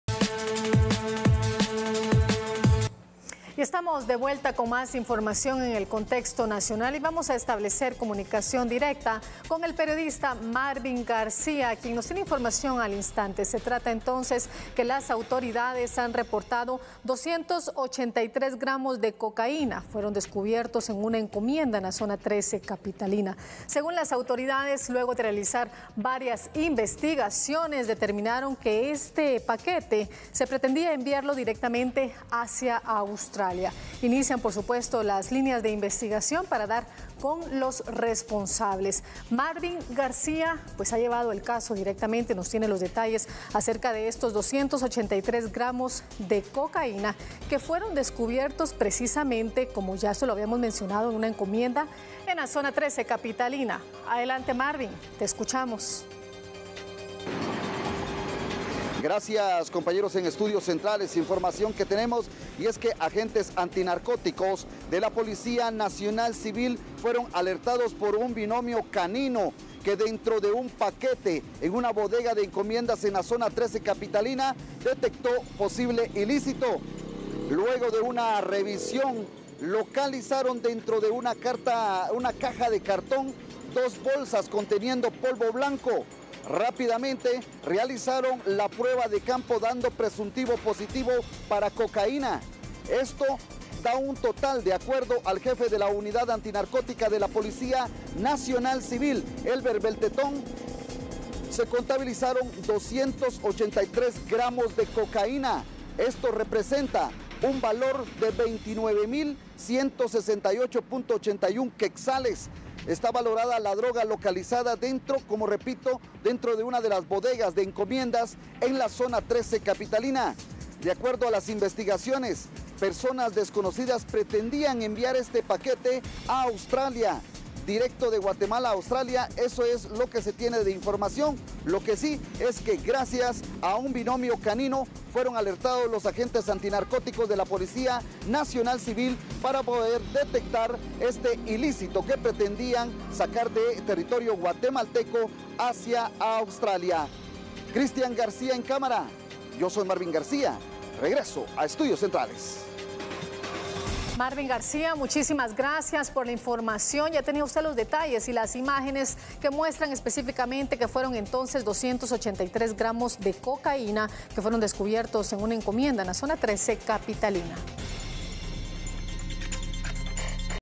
Encuentran paquete de droga que se iba a enviar a Australia
Autoridades encontraron 283 gramos de cocaína en una encomienda que estaba siendo enviada a Australia.